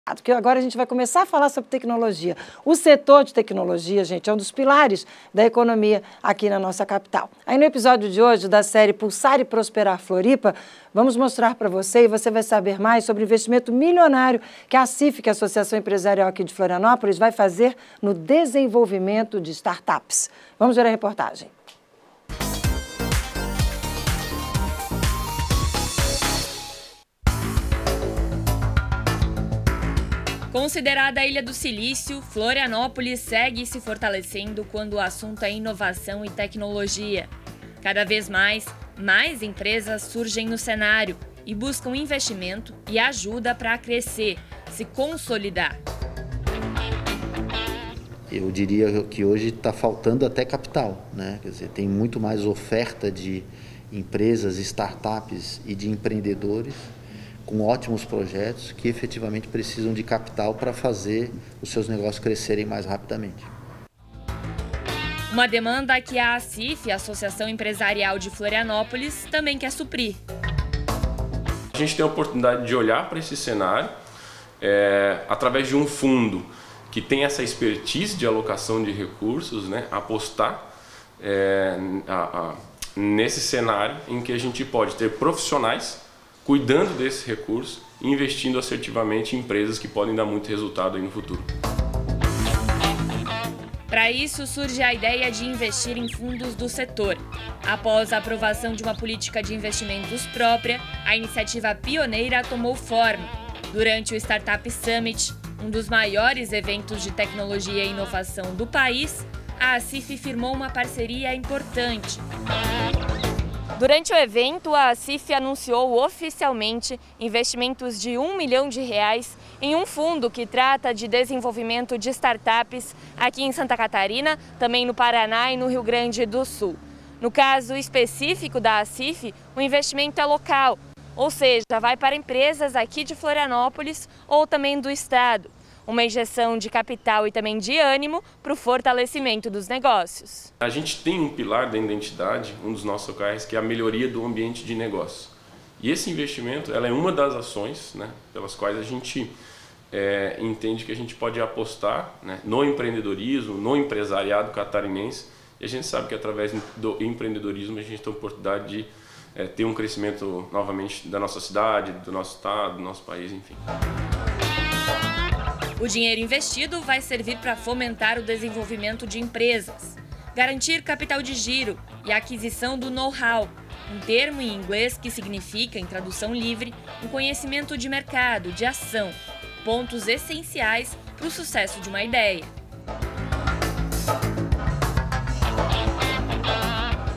ACIF anuncia investimento milionário em fundo para desenvolvimento de startups
O setor de tecnologia é um dos pilares da economia em Florianópolis. E na série “Pulsar e Prosperar Floripa”, vamos saber mais sobre o investimento milionário que a ACIF, a Associação Empresarial de Florianópolis, vai aplicar no desenvolvimento de startups.